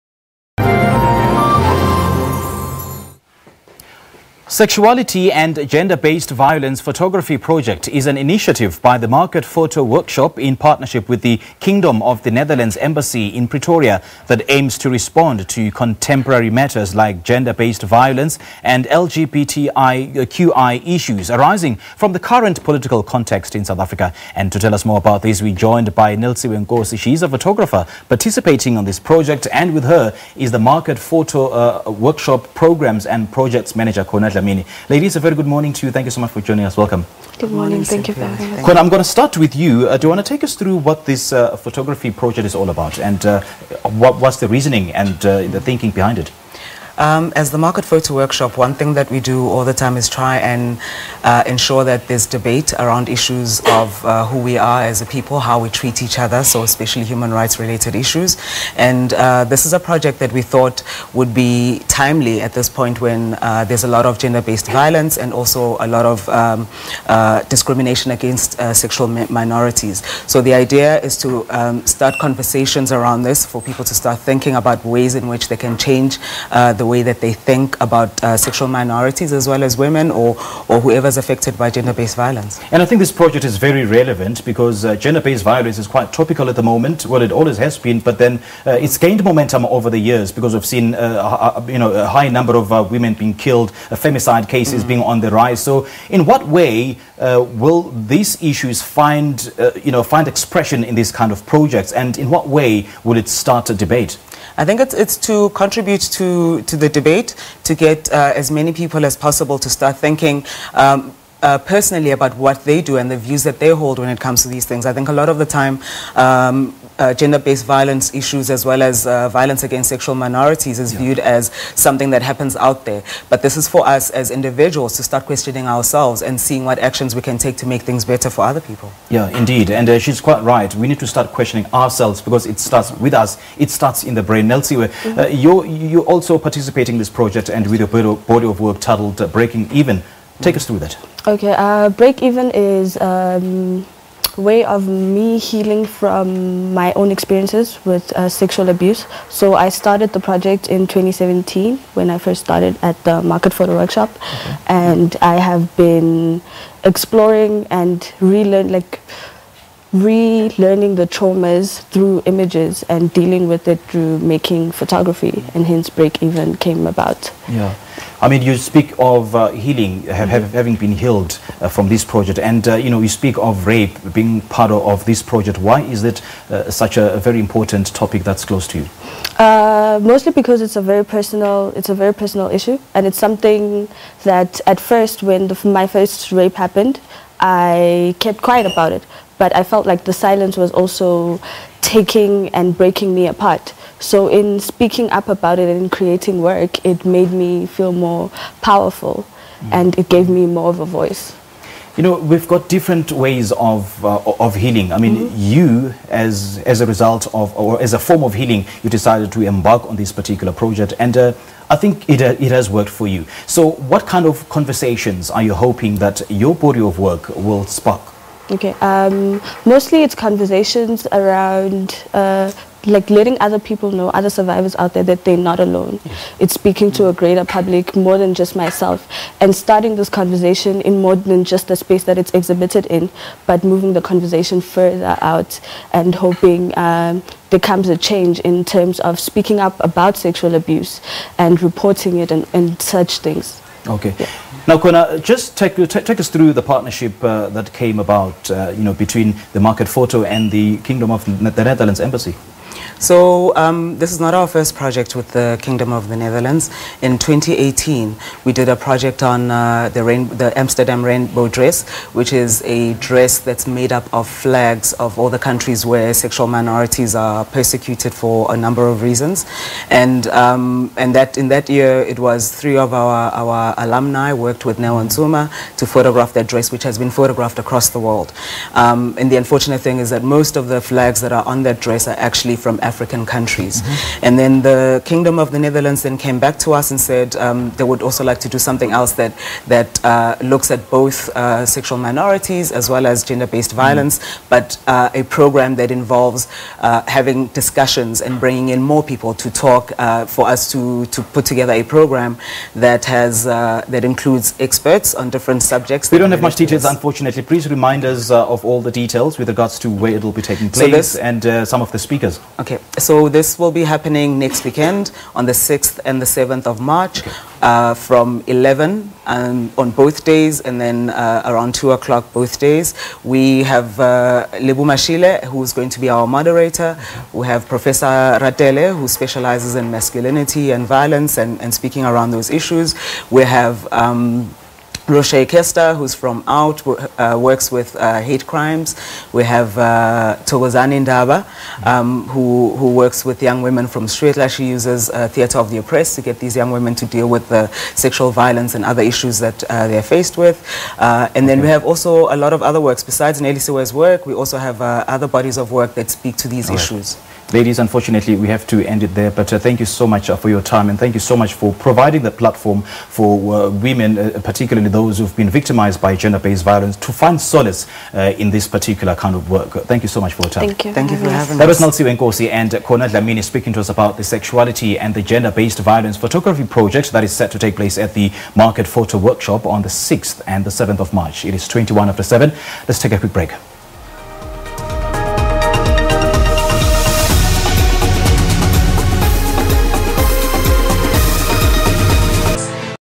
Market Photo Workshop project to tackle GBV and LGBTQI issues
SEXUALITY ANDGender-Based Violence PHOTOGRAPHY PROJECT is an initiative by the Market Photo Workshop in partnership with the Kingdom of the Netherlands Embassy in Pretoria that aims to respond to contemporary matters like Gender-Based Violence and LGBTQI issues, arising from the current political context in South Africa. and to tell us more about this, We are now joined by Nelisiwe Nkosi, she is a photographer participating on this project, and with her is the Market Photo Workshop's programs and projects manager Khona Dlamini.